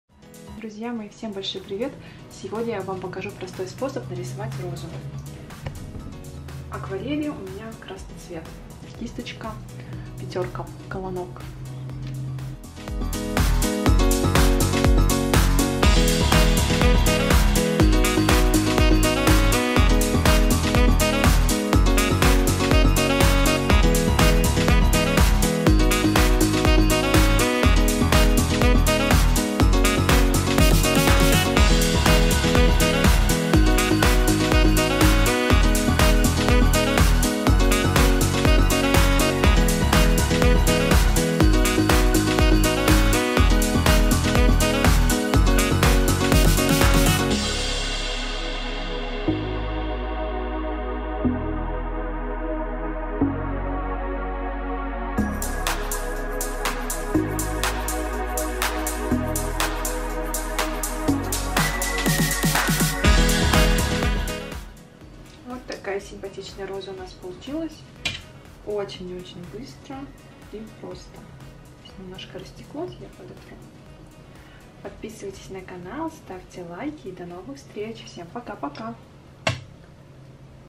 КАК НАРИСОВАТЬ РОЗУ / БЫСТРО/ ПРОСТО /КРАСИВО
Простой урок "как нарисовать розу быстро" с помощью акварели
Это очень просто и быстро!